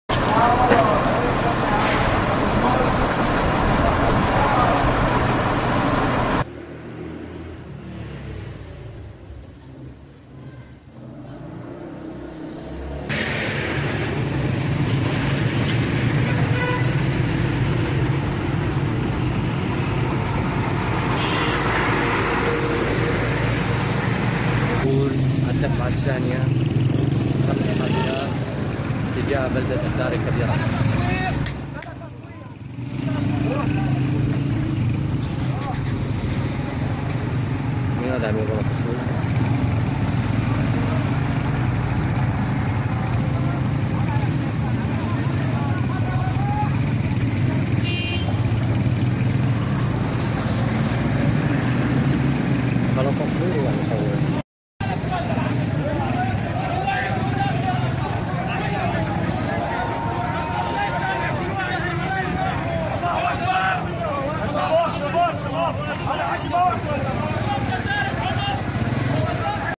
Raw Syrian Rebels Evacuate Strongholds in Homs
last minute news doruksa-news